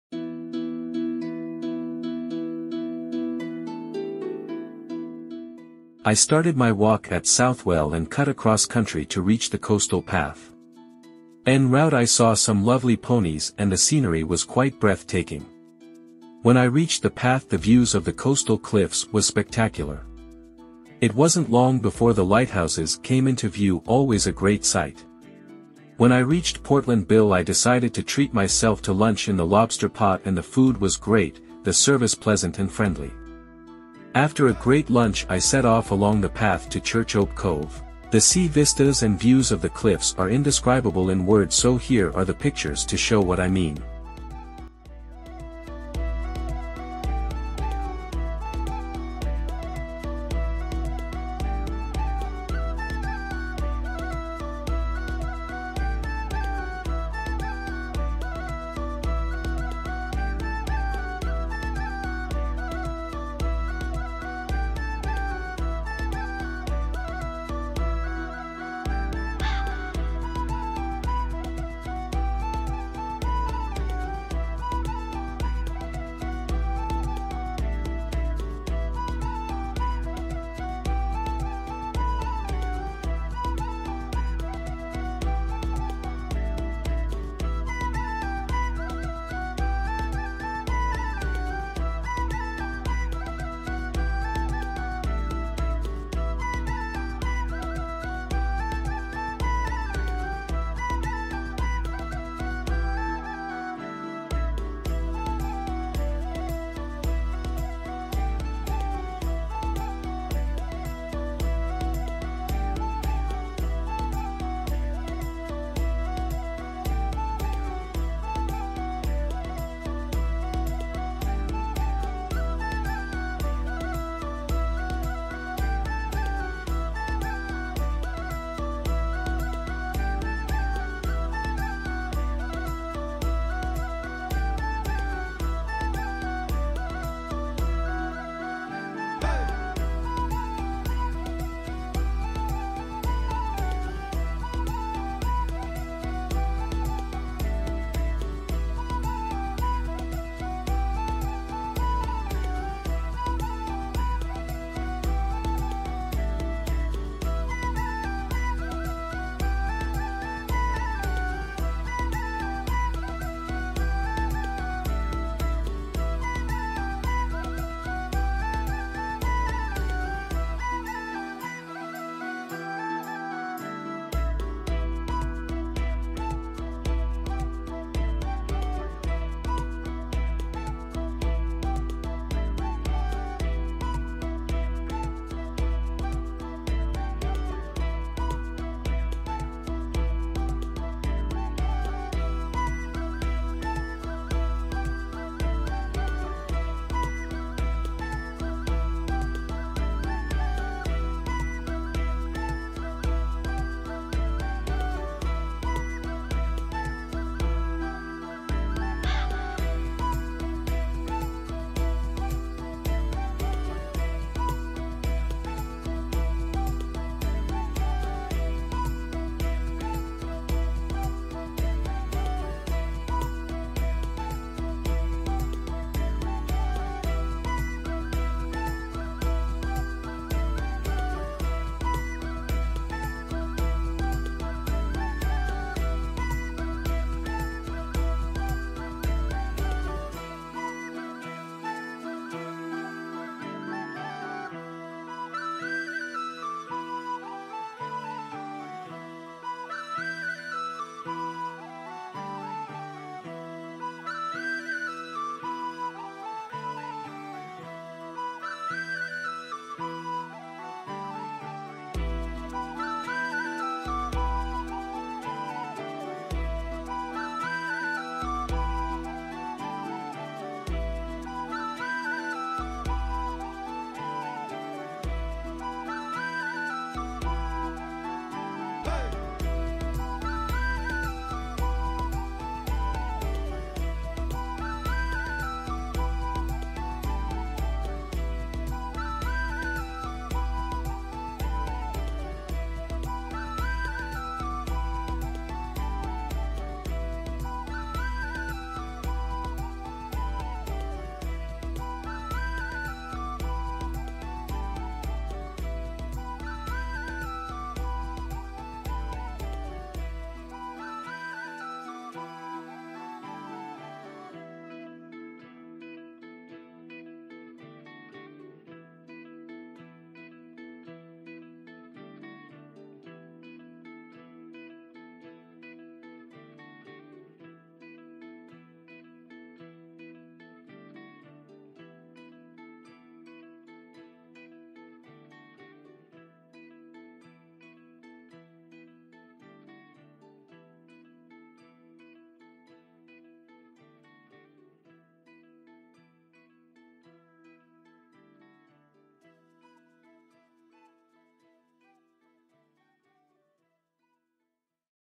Walk from Portland Bill
Walking from Portland Bill to Church ope Cove